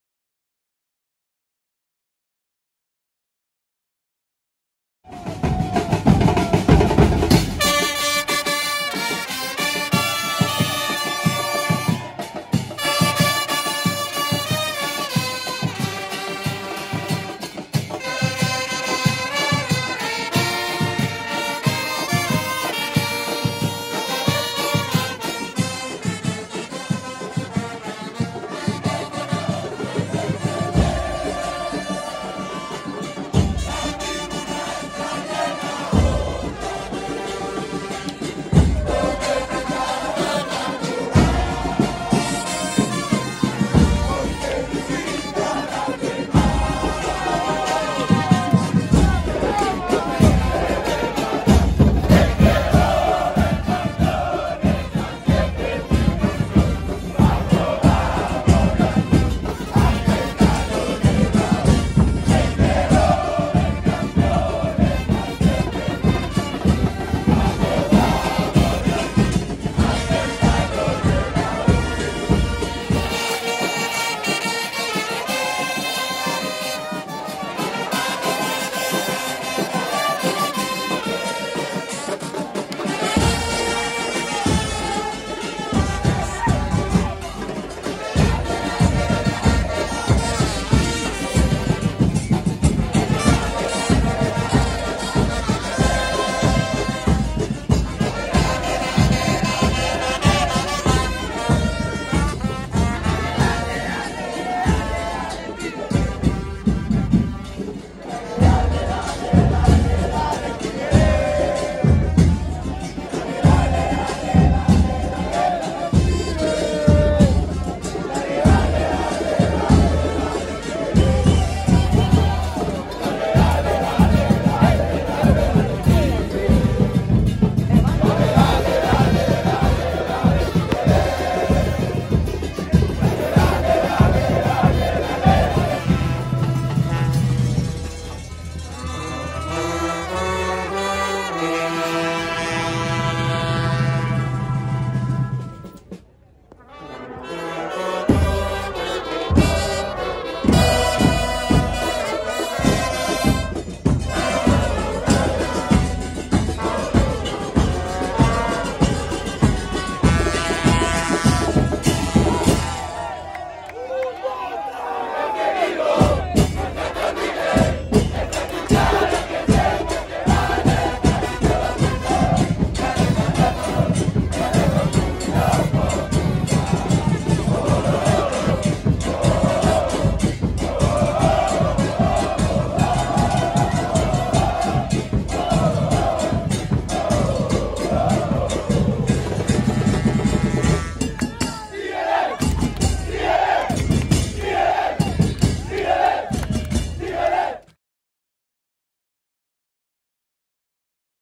EL COLOR DE LOS LIBRES Y LOKOS RUMBO AL CLÁSICO REGIO🔵🟡
En este video, te dejo la mini-cobertura que realizamos al banderazo por la paz de la barra de los Tigres, los Libres y Lokos, en los alrededores de la Flama Universitaria, minutos antes del inicio del Clásico Regiomontano 127.

Algunos elementos, entre ellos el líder de la barra, Samuel Reyes, portaron camisetas rojas con la  leyenda: "QUEREMOS IR, QUEREMOS QUE VENGAN".

-Facebook: Gilberto Galván Quirino (página)